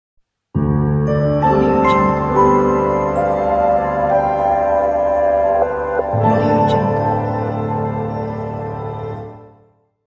Atlas Tourism Logo Animation
here is an another logo animation video made for Cocktail- Mix up Imaginations hope you like it